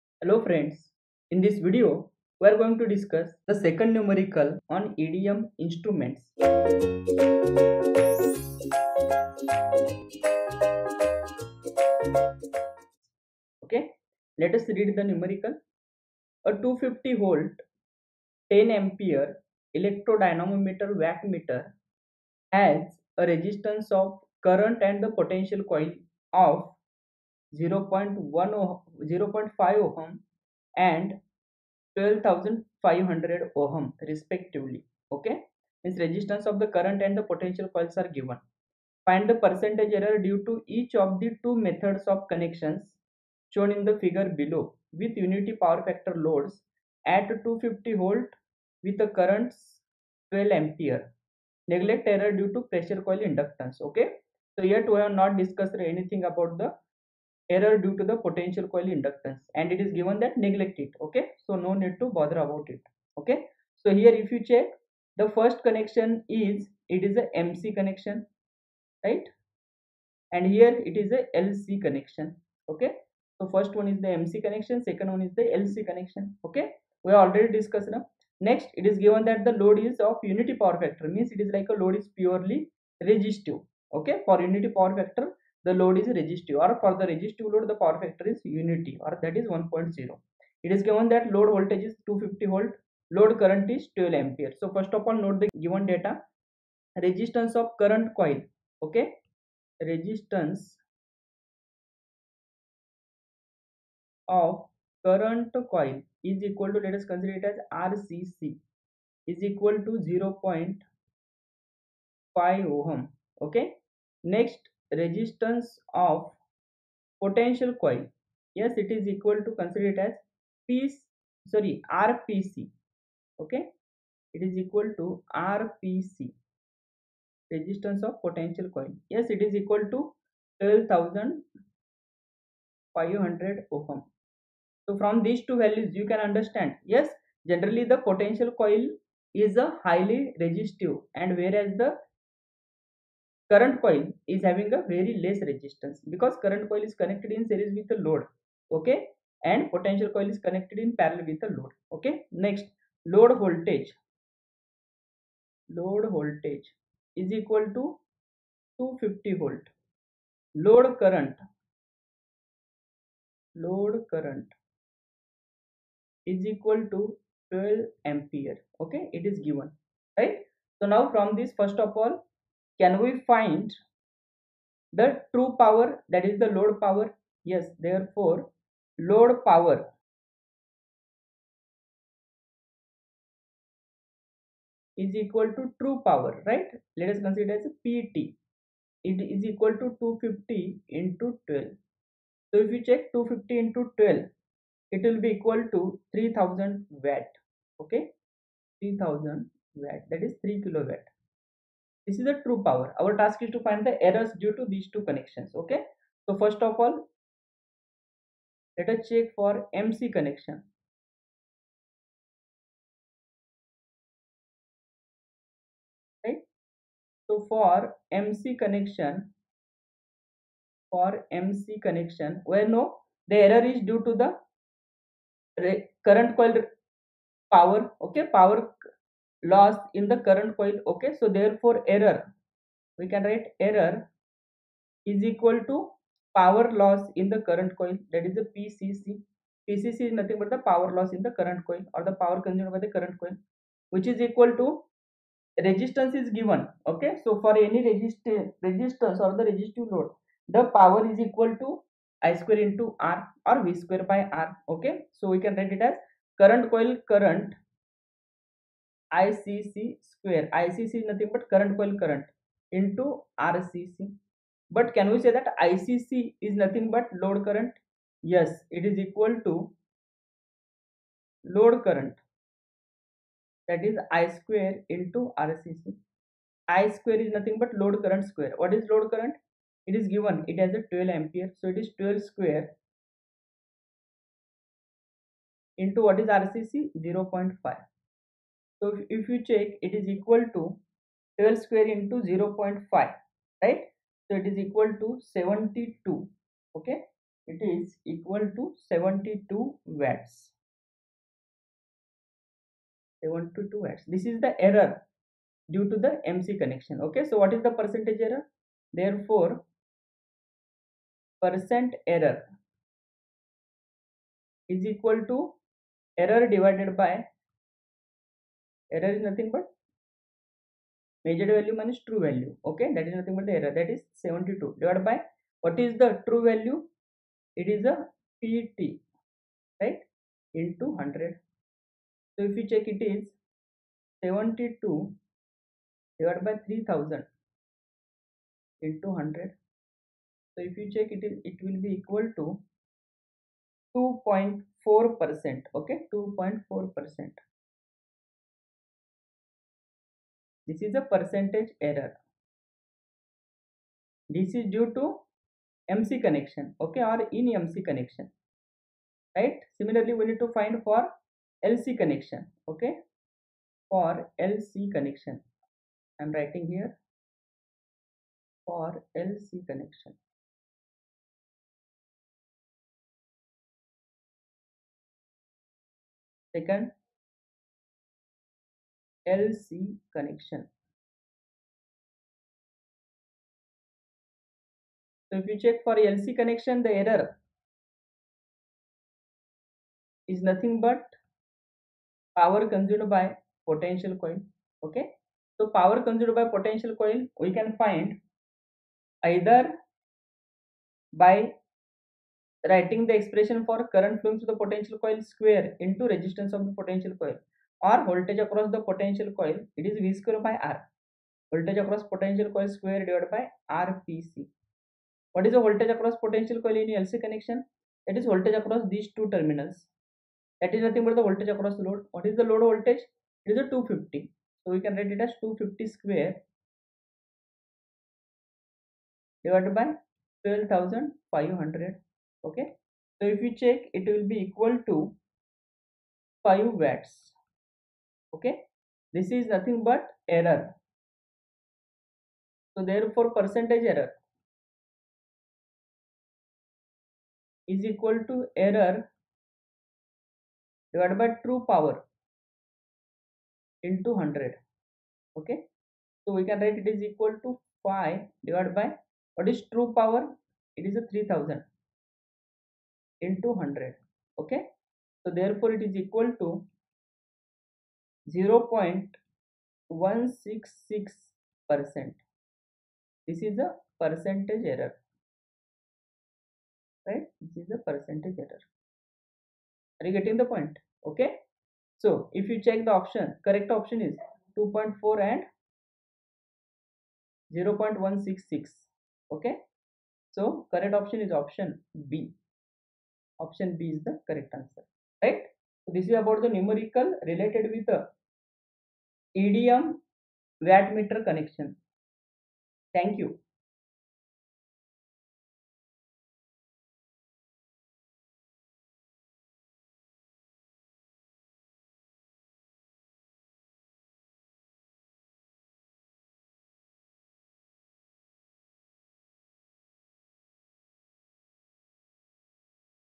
Numerical 2: EDM Instruments | Electromechanical | GATE Electrical and Electronics Measurement
Explore the practical side of Electromechanical Devices and Measurements in GATE Electrical and Electronics! Dive into "Numerical 2: EDM Instruments," delving into intricate details, problem-solving, and real-world applications. Unravel the complexity of instruments, understanding their role in electrical measurements. Join us to enhance your knowledge and proficiency in this essential aspect of the GATE syllabus. Don't miss this opportunity to grasp concepts crucial for success!

Happy Learning.